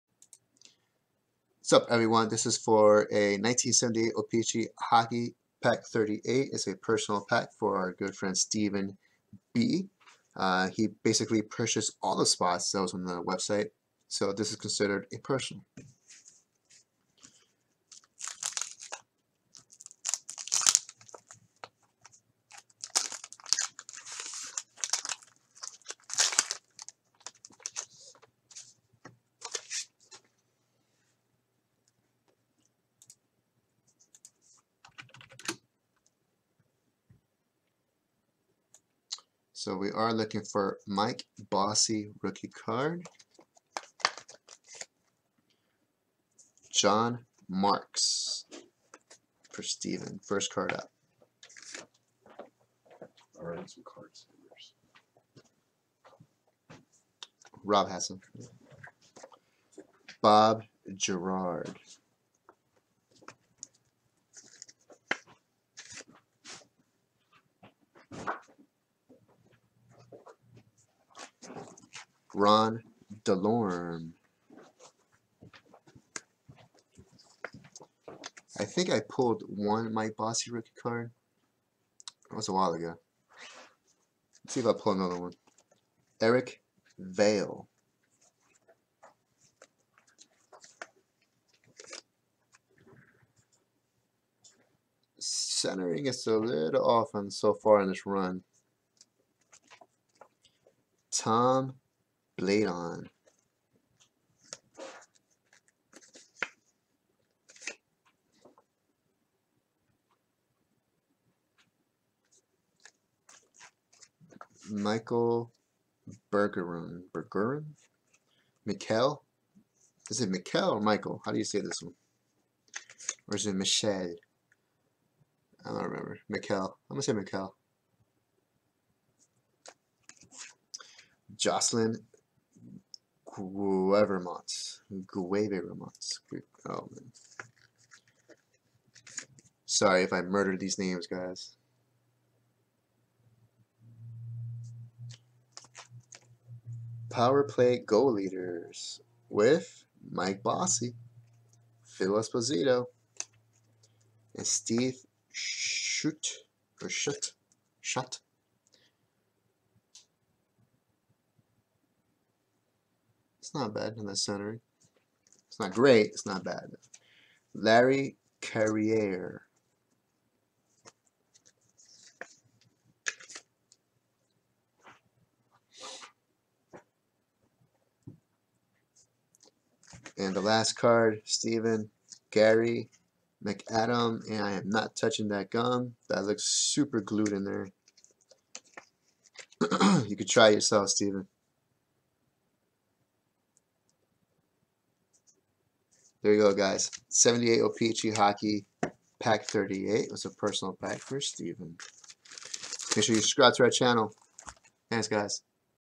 11-16-18 1978 O-Pee-Chee Hockey Wax Pack 38 Break Video Opening
1978 O-Pee-Chee Hockey Wax Pack 38 Break Video Opening